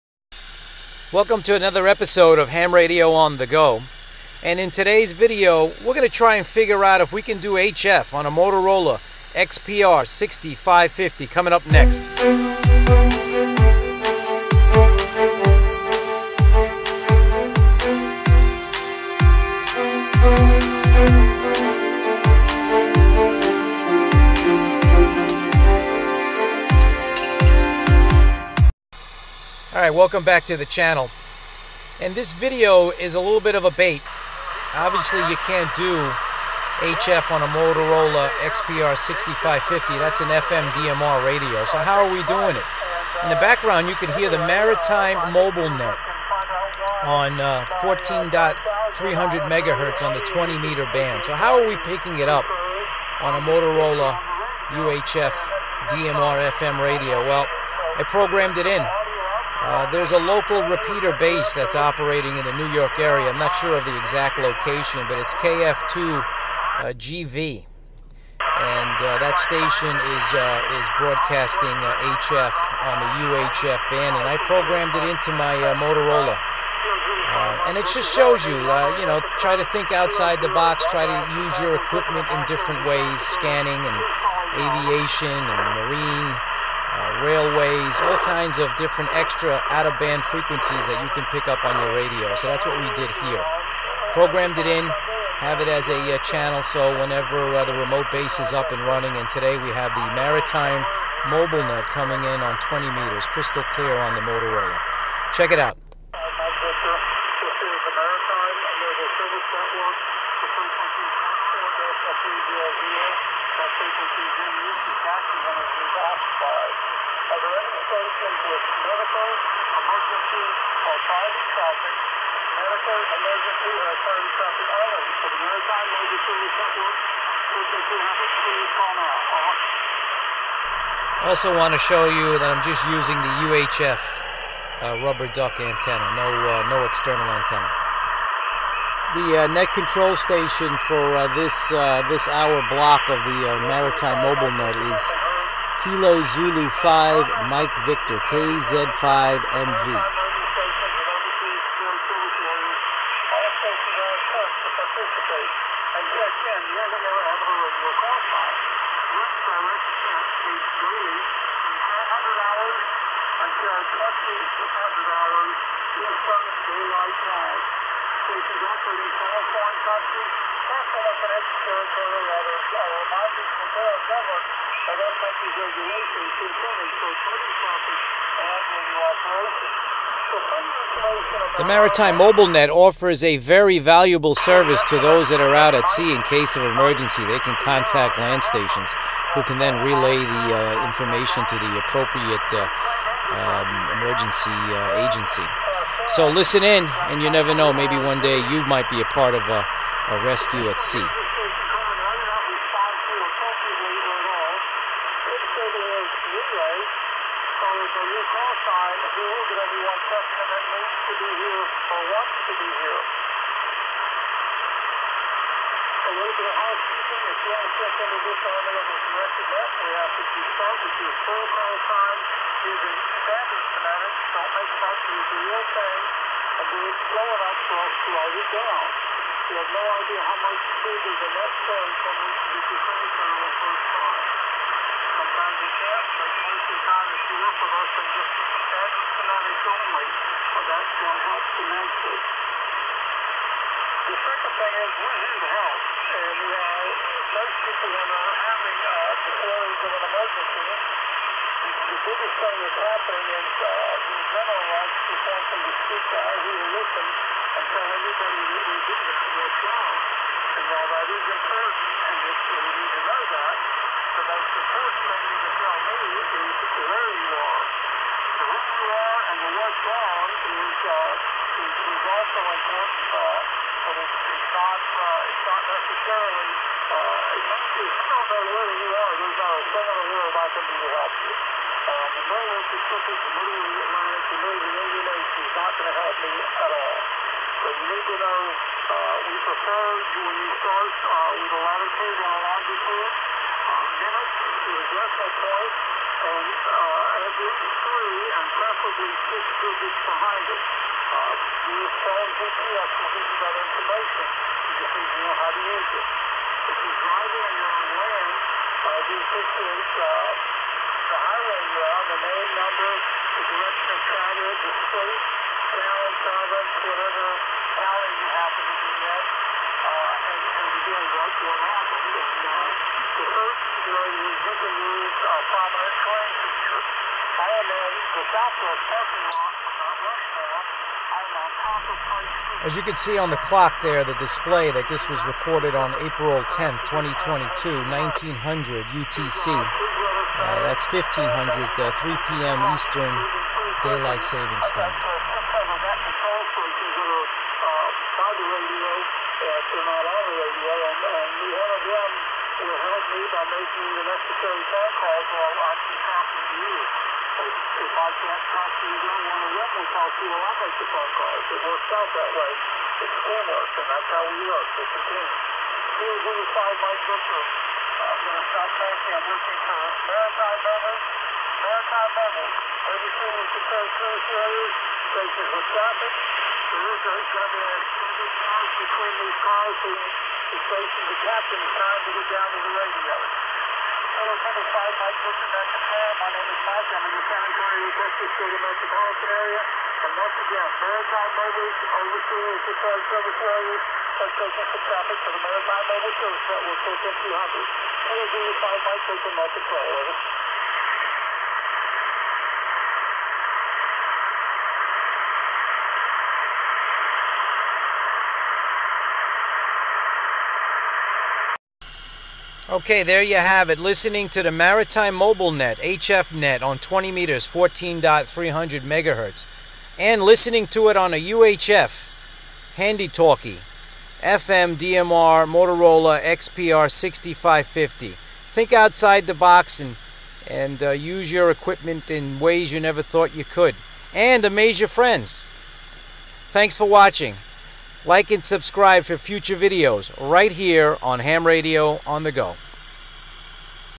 HF on a Motorola XPR 6550 DMR FM HT? 20M Maritime Mobile Net 14.300 mhz. KF2GV KZ5MV
Can you go on HF with an UHF FM DMR HT? No, not that I know of but before I get some saying that its a bit of bait and its true to an extent but I wanted to show how we can use existing equipment in new ways to get more out of our equipment especially if your on a budget.

So lets listen in on the Maritime Mobile Net on a Motorola XPR 6550.